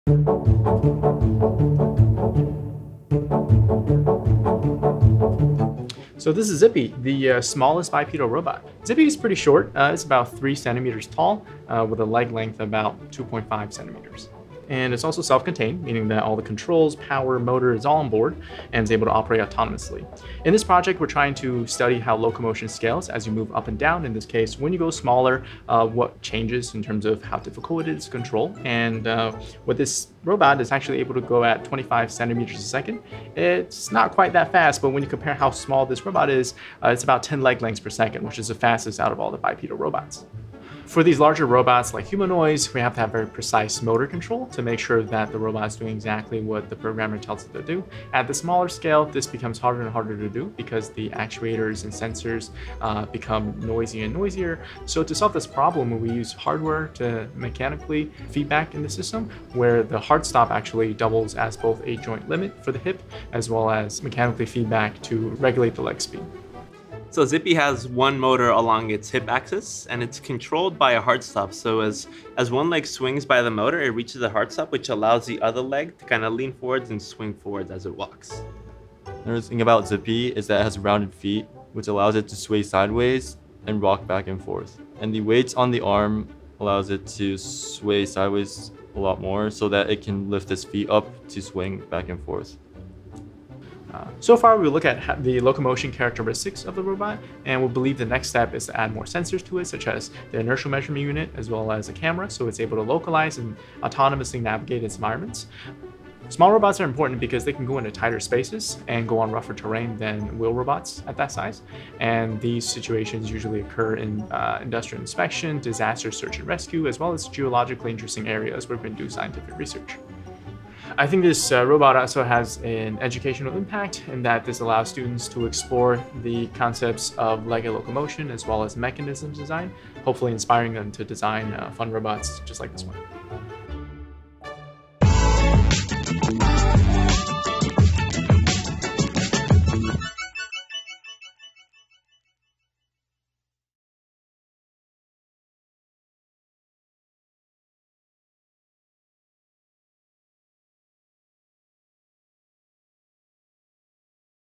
The world's smallest, power-autonomous bipedal robot
At only three centimeters tall, Zippy, the world's smallest bipedal robot, is also self-contained--all the controls, power, and motor are on board so that it operates autonomously. Moving at 10 leg lengths per second, it is also the fastest bipedal robot.